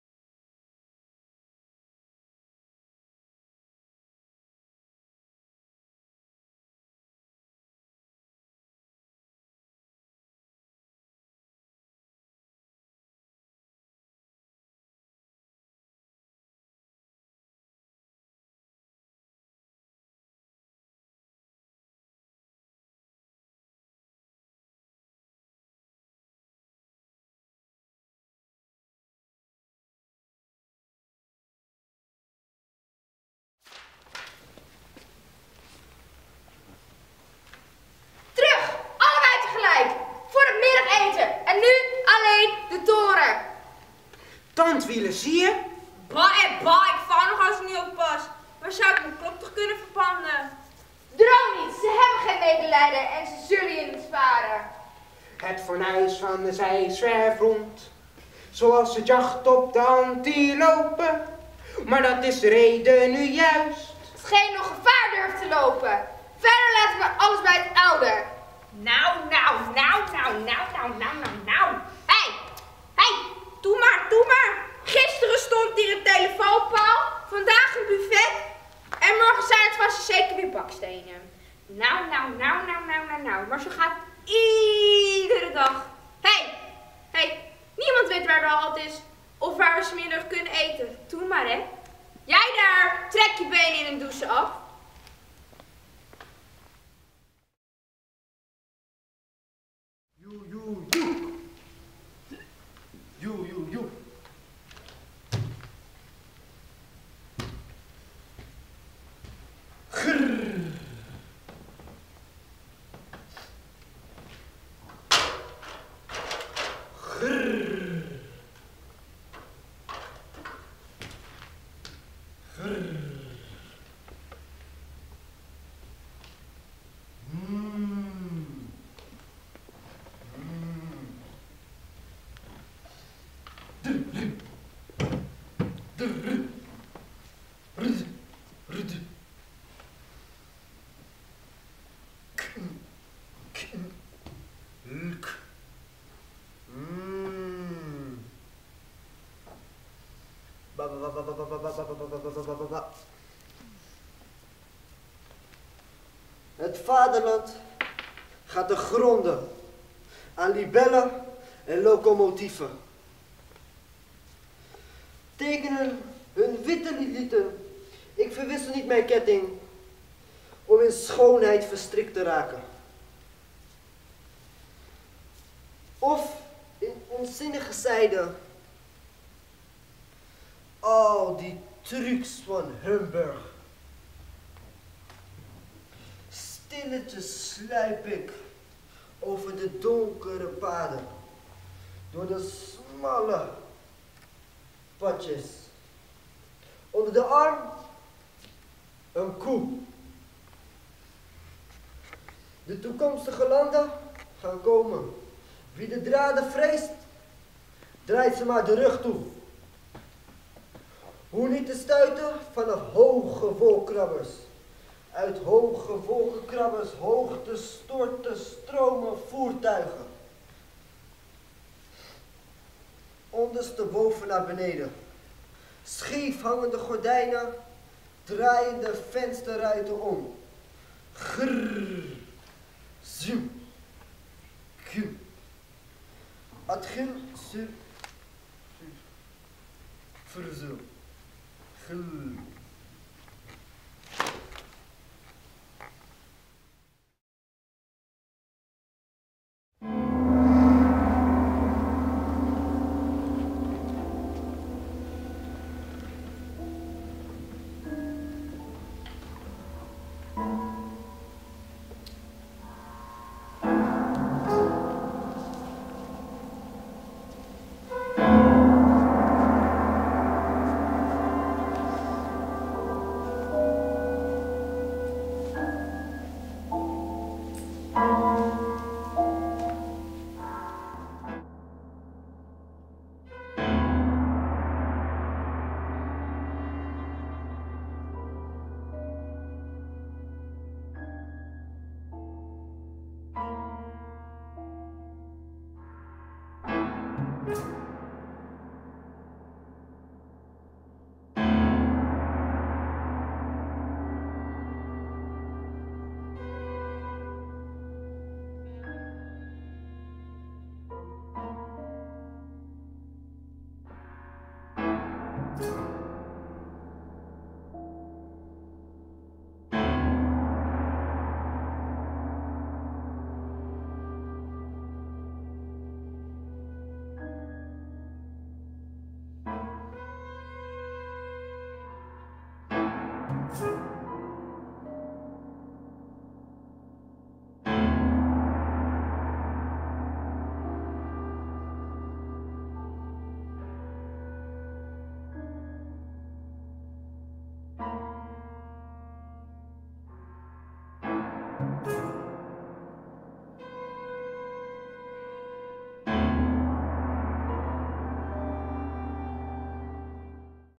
VOTS2 28nov16 dl3
derde deel van doorloopregistratie.Scenes worden vrijdag 12 december opnieuw opgenomen met toevoeging van laatste nog niet opgenomen sacenes in doorloop.